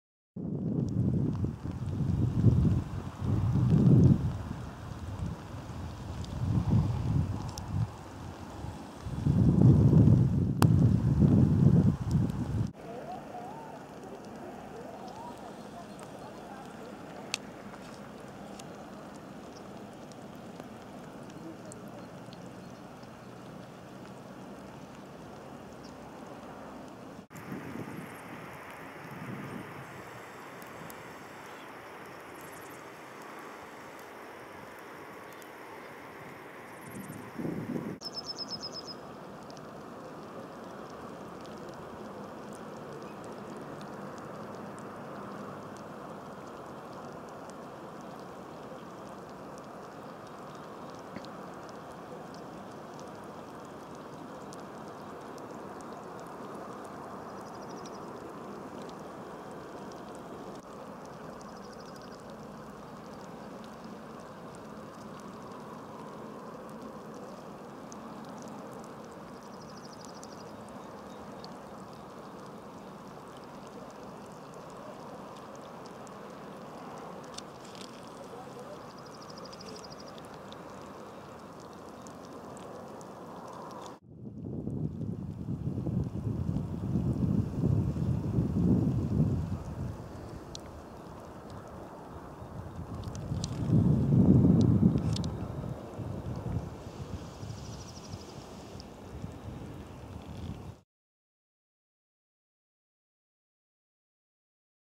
Masskläckning av bäcksländor
Bäcksländor av olika arter har kläckts i stora mängder i Skellefteälven sista dagarna. 15 april 2013.